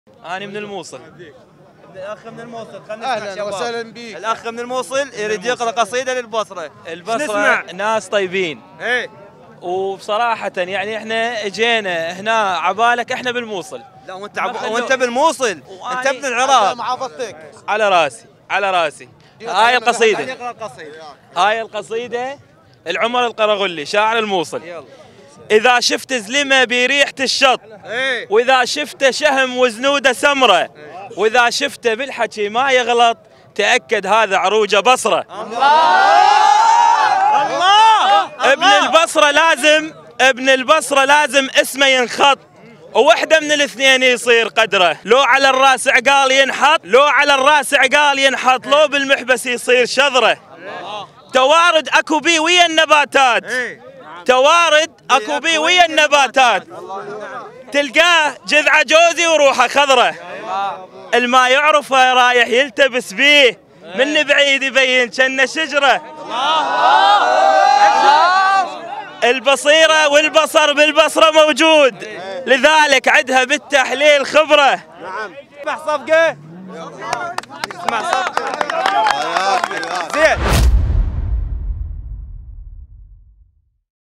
عند الكورنيش.. عراقي من الموصل يقرأ قصيدة لأهل البصرة اثناء زيارته لخليجي 25
كأس_الخليج مع علاوي احمد